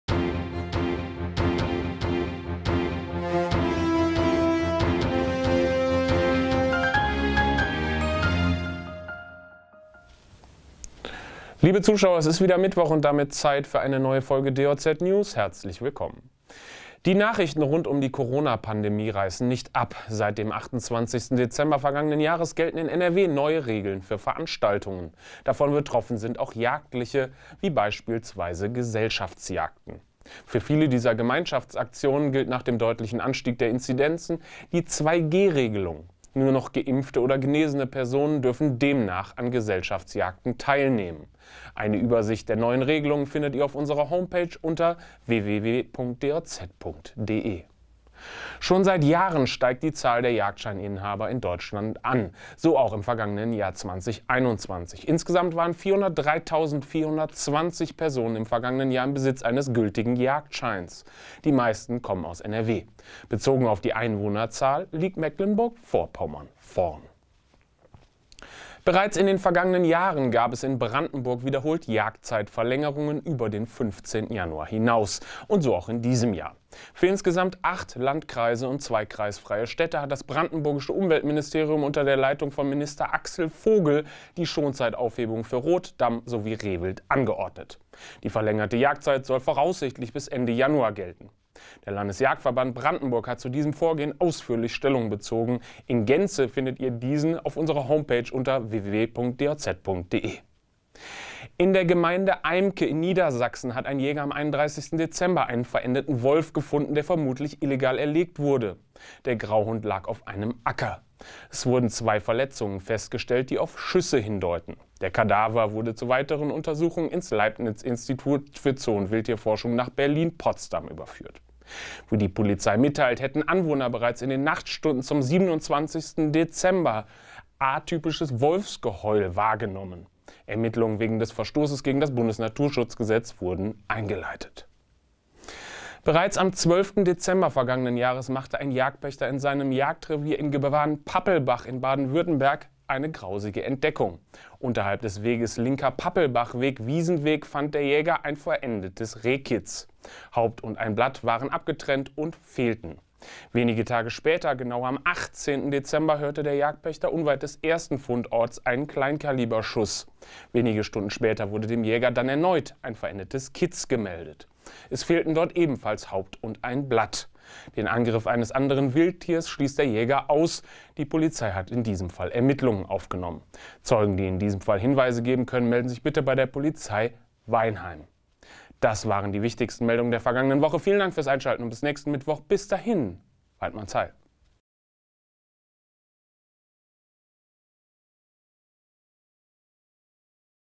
2G bei Gesellschaftsjagden, Verlängerung der Jagdzeit, viel mehr Jäger u.w. - DJZ NEWS 1/2022
In dieser Folge: So viele Jäger wie noch nie, 2G bei Gesellschaftsjagden, Verlängerung der Jagdzeit in Brandenburg, Jäger meldet Wolfs-Kadaver und Wilderei bei Schriesheim.